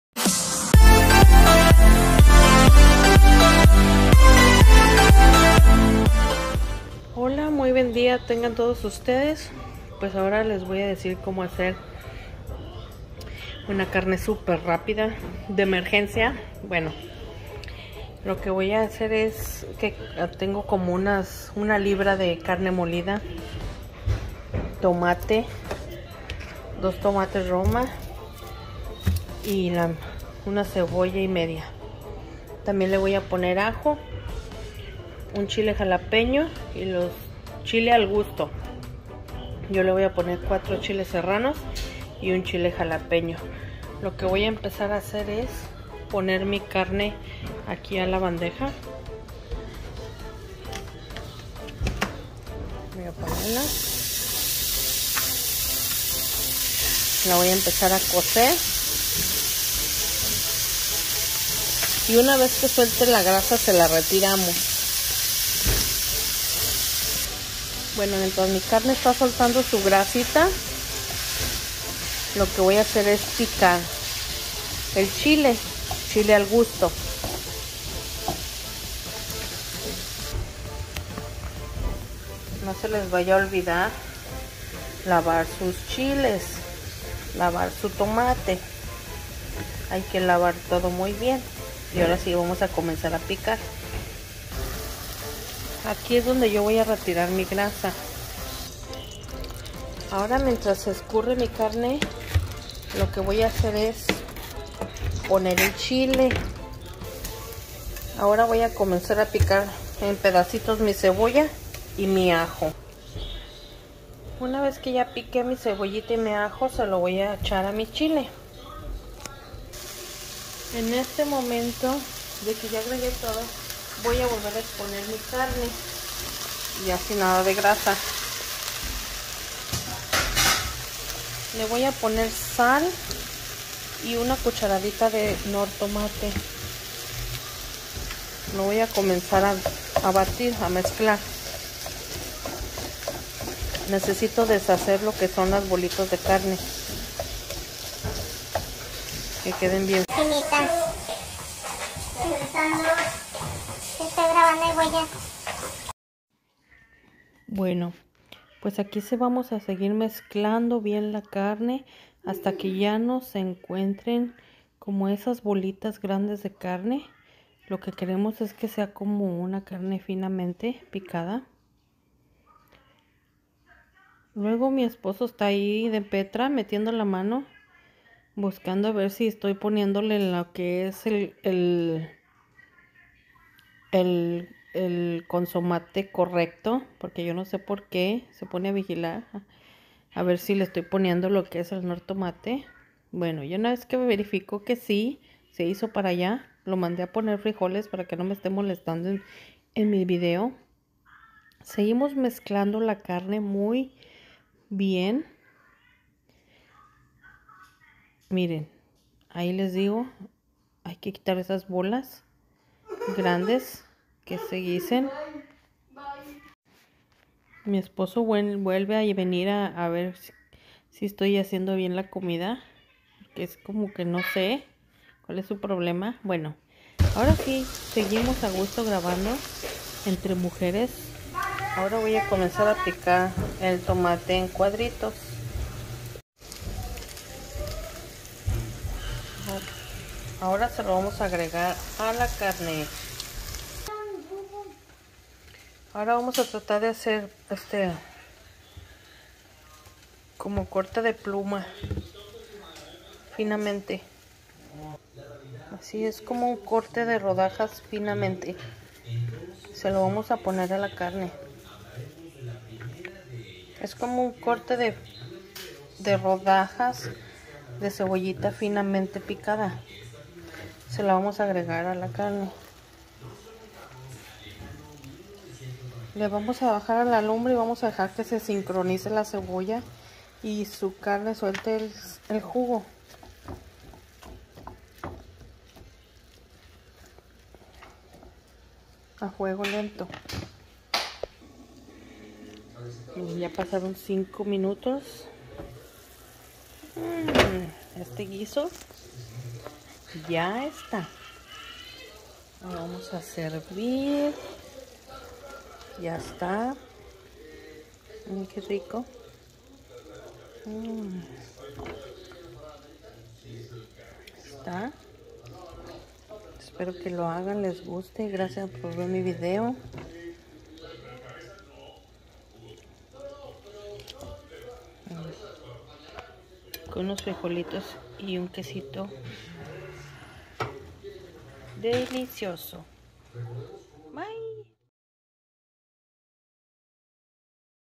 como hacer carne molida chile, tomate y cebolla
una libra carne molida
dos tomates
una cebolla y media
un ajo
knor tomate